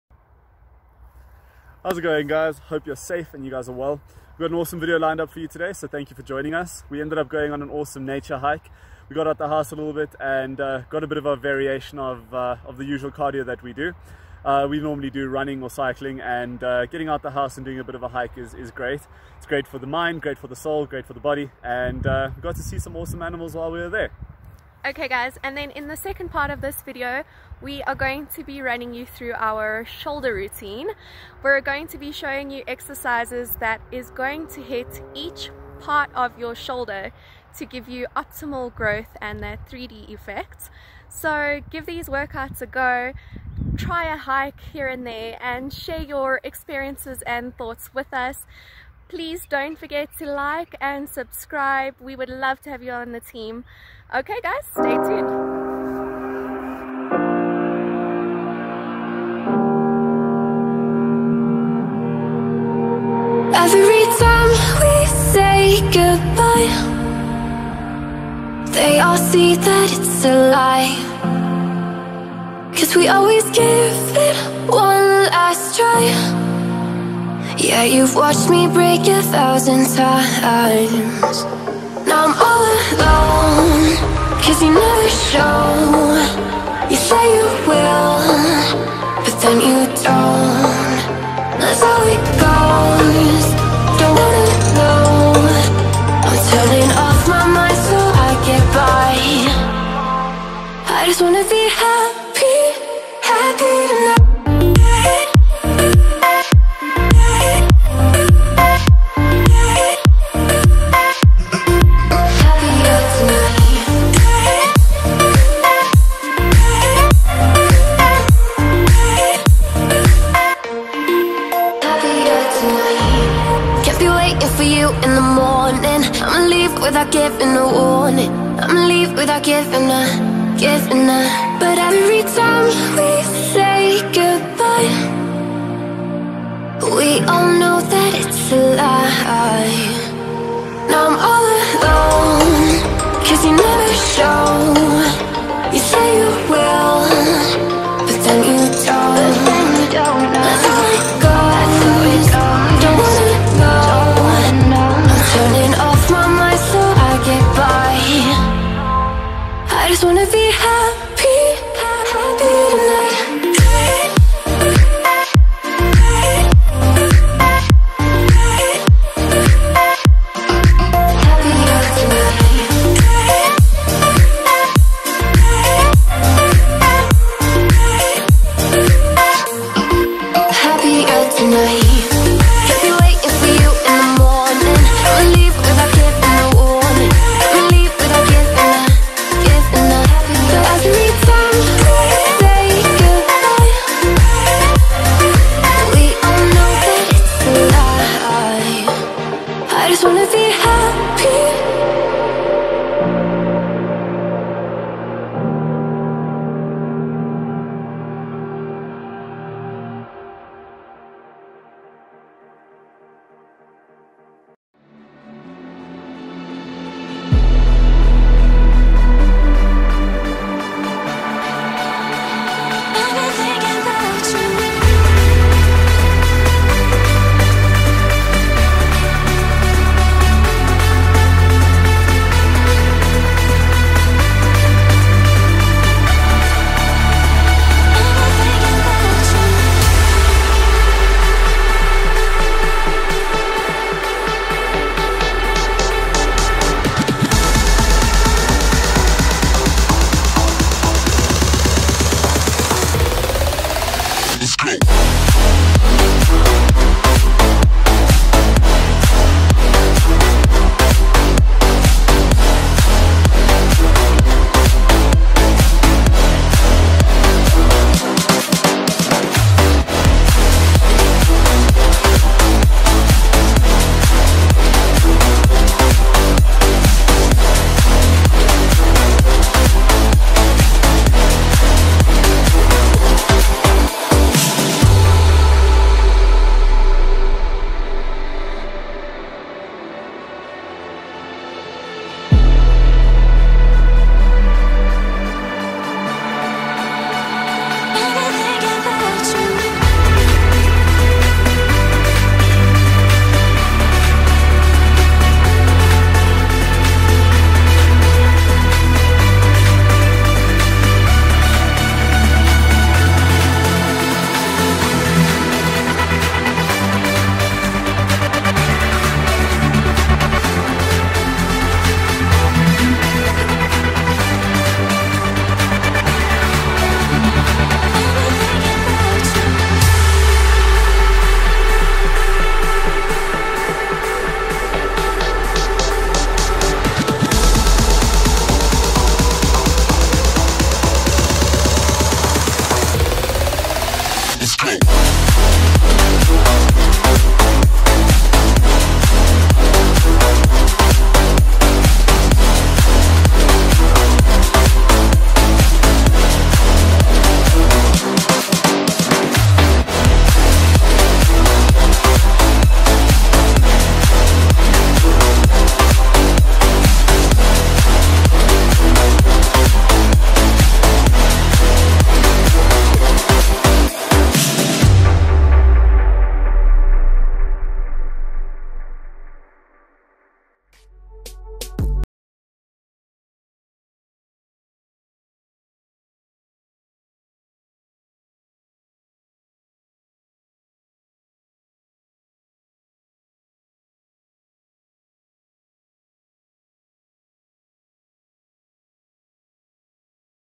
The most epic safari hike followed by our Shoulder Blaster!
We had an amazing time hiking out in the wilderness and were fortunate enough to have some up close and personal encounters with some of the local wildlife.
Join us for an even more amazing shoulder workout which will leave you feeling pumped!